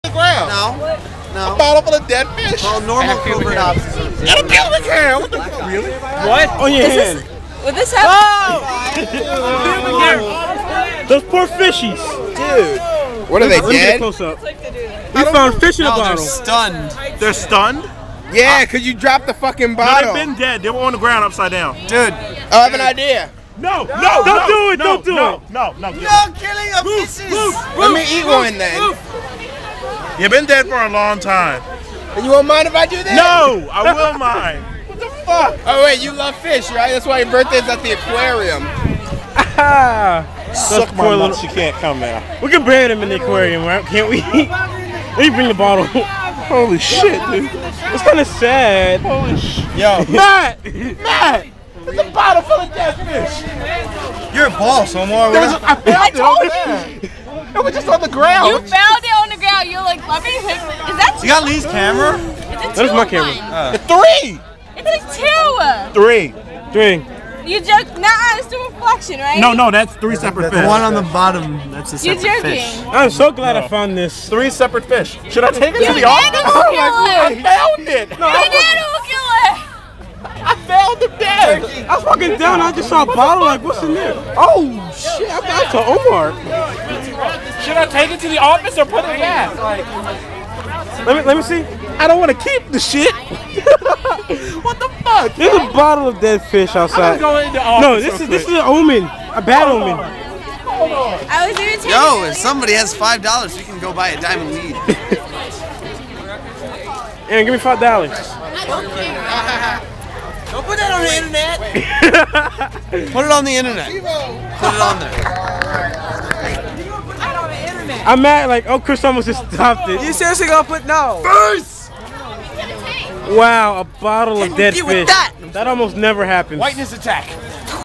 Fish In Bottle
i found a random bottle on the side of the walkway at school, turns out theres 3 dead fish in there. crazy random